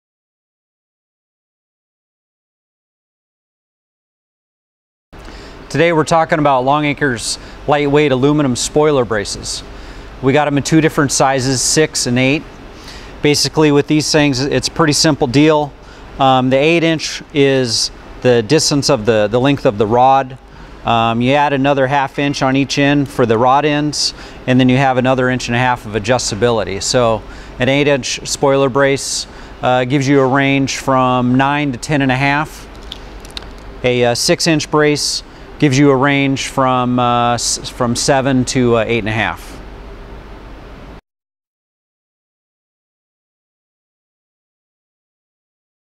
Longacre Spoiler Supports
Gary Lewis gives us a quick overview of the Longacre spoiler support braces and the lengths that are available.